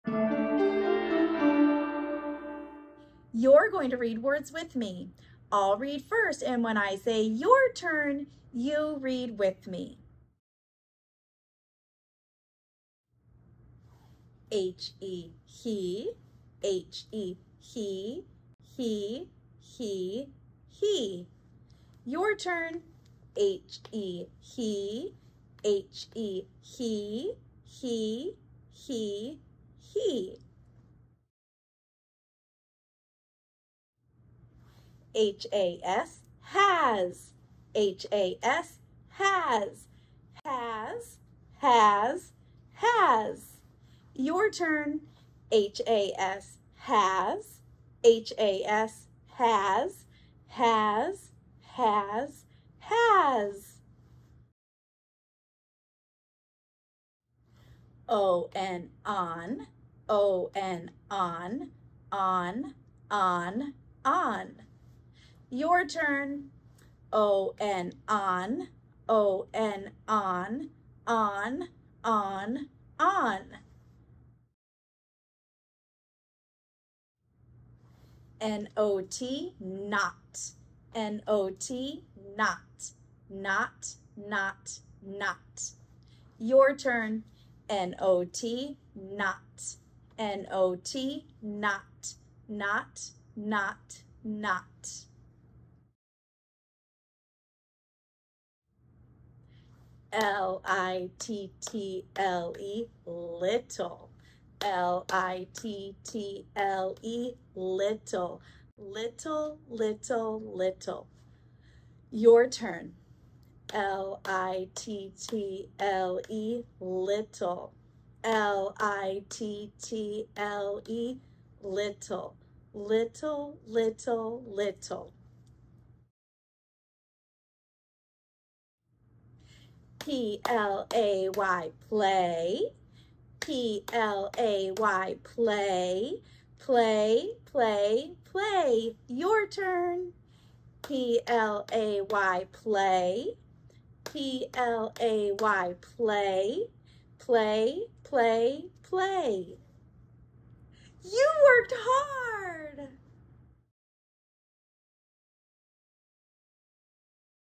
High-Frequency Words: Unit 4
Bloomington Kindergarten teachers created a set of learning videos in order to meet the needs of our youngest learners. Each video highlights letters, sounds, numbers, or words that we want children to know by the end of their kindergarten experience.

HFW Unit 4 focuses on six high-frequency words: He, Has, On, Not, Little, Play